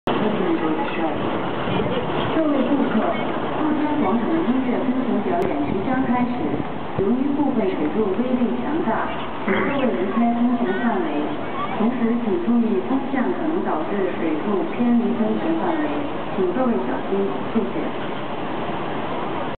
東涌東薈城音樂噴泉(表演前廣播)
東涌東薈城音樂噴泉日期:2009年9月12日 下午4:00
廣播語言:普通話
Tung Chung Citygate Musical Fountain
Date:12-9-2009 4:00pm
broadcasting language:Pu Tong Hua